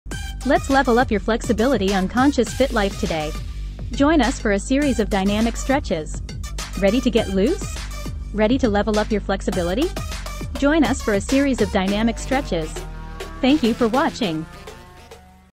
Overhead Tricep Extension Your Upper Body Toner Conscious Fit Life
"Tone your upper body with the overhead tricep extension. Our new video will guide you through this effective exercise.